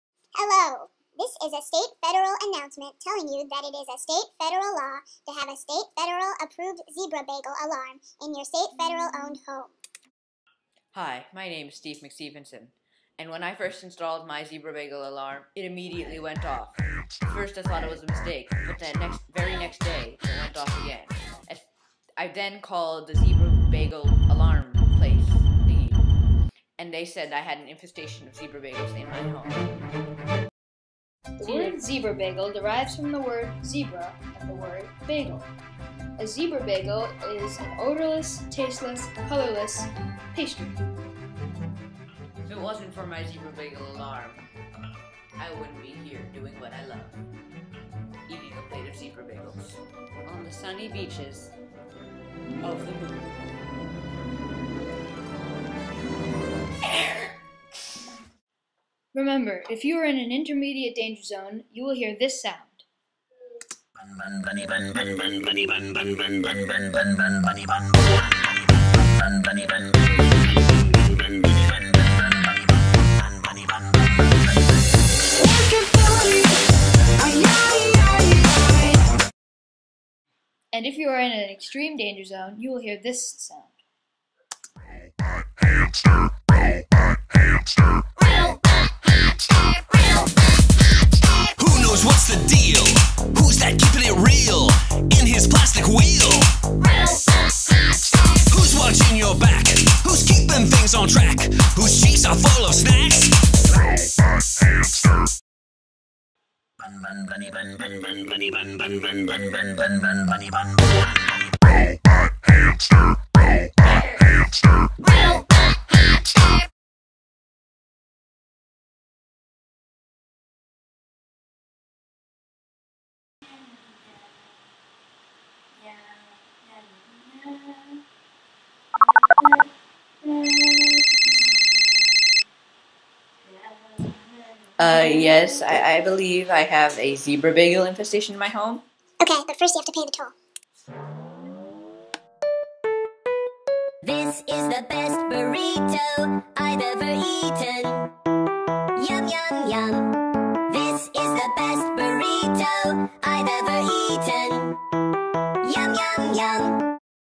Zebra Bagel Warning Commercial
This is a State-federal announcement telling you it is a State-federal law to have a state-federal approved Zebra Bagel Alarm in your State-federal owned home.